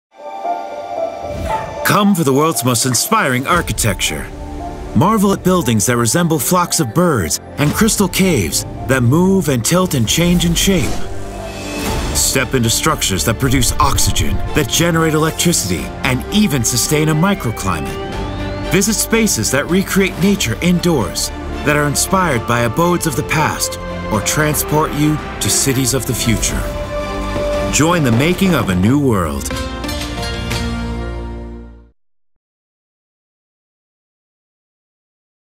Inspiring Architecture
Explore the city of the future and discover the world’s most visually striking and emotionally inspiring architecture, every day for six months @expo2020dubai